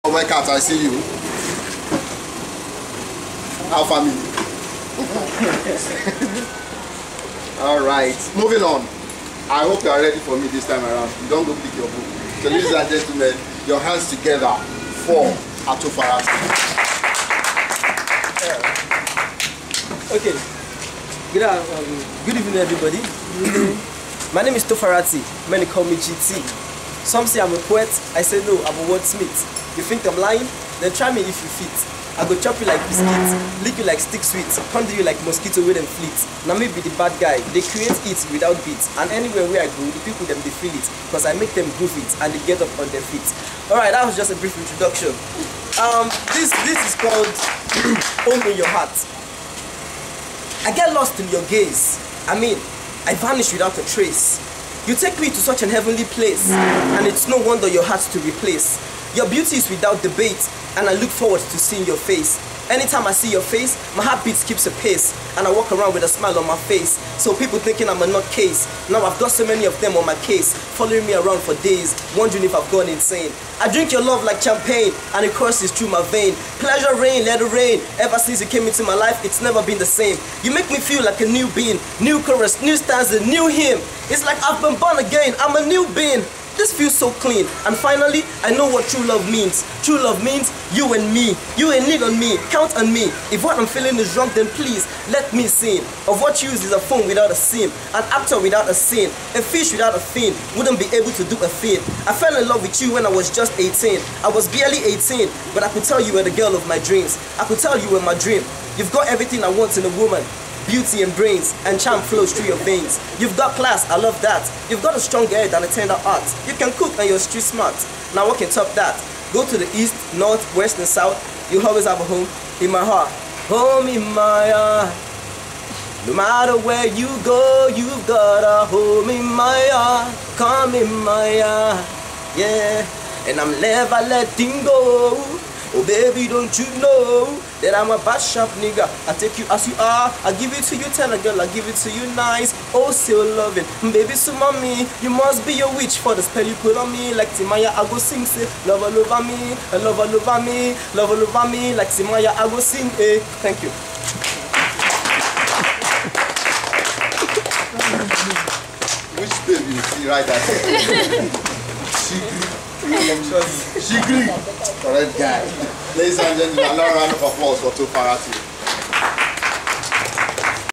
Spoken Word Poetry by Tofarati titled, Home in my heart, at Chill and Relax (October Fever)
Spoken Word Poetry by Tofarati titled, Home in my heart, at Chill and Relax (October Fever) on October 14, 2012 in Gbagada, Lagos, Nigeria. Chill and Relax is a monthly open mic Performance Poetry, Spoken Word and Soul Music Event.